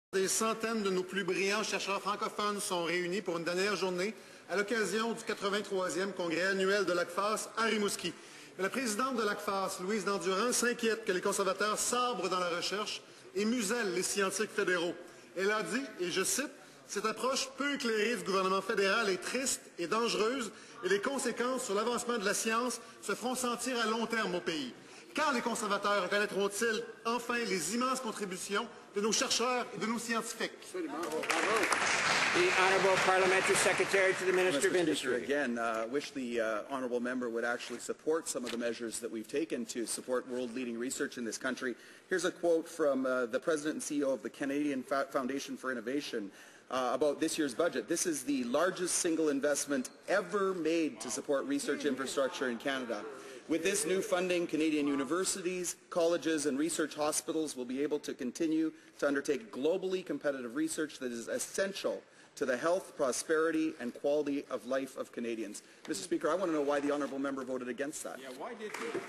Question de Guy Caron sur le musellement des scientifiques
En lien avec le congrès de l'ACFAS qui se tenait à Rimouski, j'ai questionné le gouvernement sur les coupes en recherche, le musellement des scientifiques et l'importance de reconnaître la contribution des scientifiques.
Période des questions du 29 mai 2015.